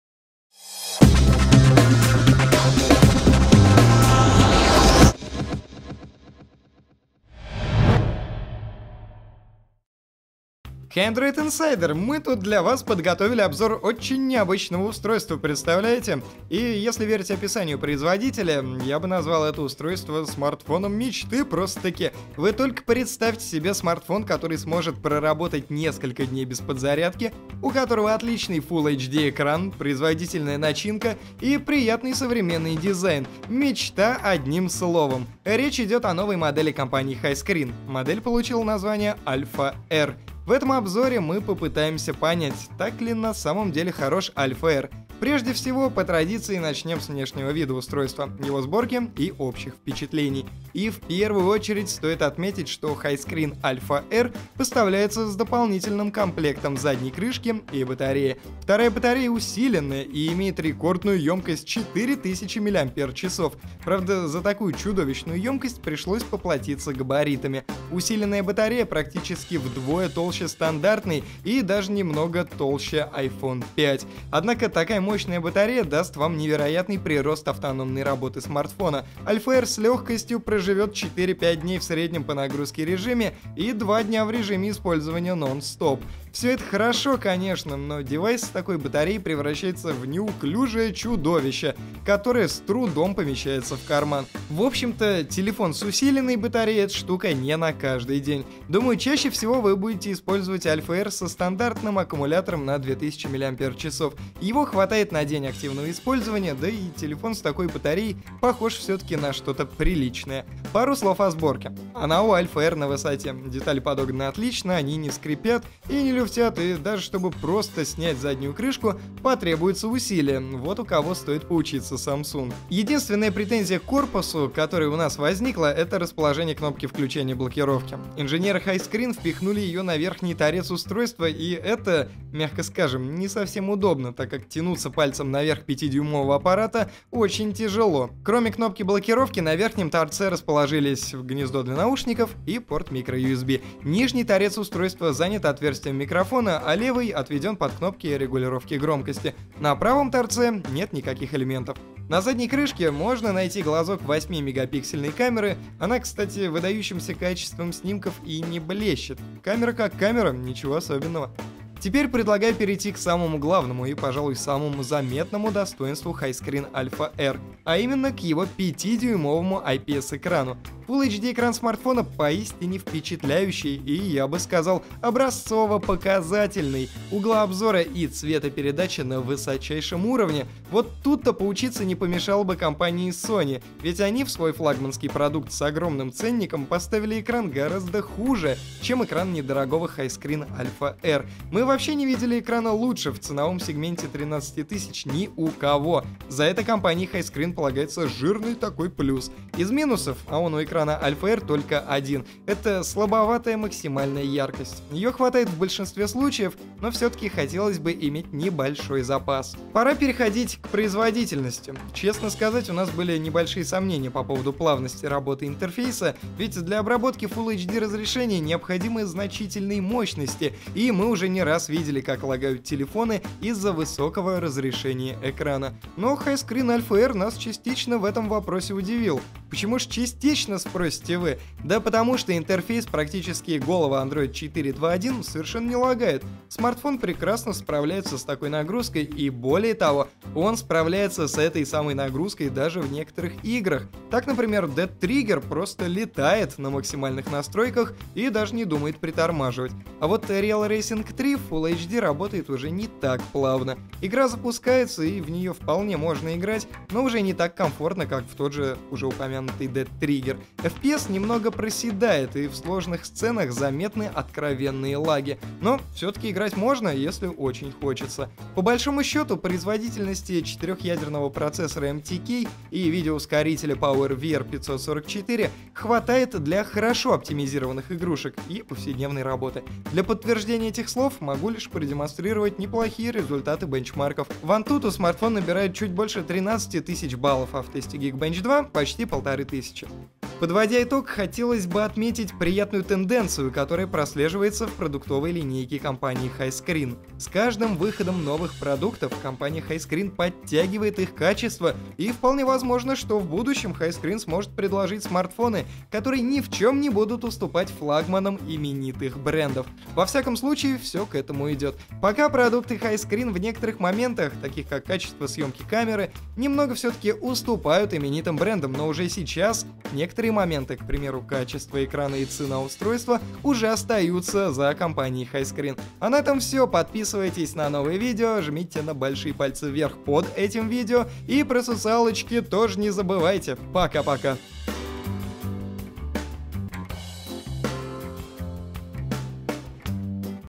Highscreen Alpha R - Самый Долгоиграющий Смартфон с FullHD экраном. Обзор AndroidInsider.ru
Прочитав описание смартфона Highscreen Alpha R, начинаешь думать, что это гаджет мечты. Посудите сами, пятидюймовый FullHD экран, выполненный по технологии IPS. Четырехядерный процессор с мощным видеоускорителем PowerVR SGX544. К тому же все это добро подпитывается от невероятно емкой батареи - 4000мА/ч. В этом обзоре мы попытались разобраться, так ли хорош на самом деле новый смартфон от Highscreen.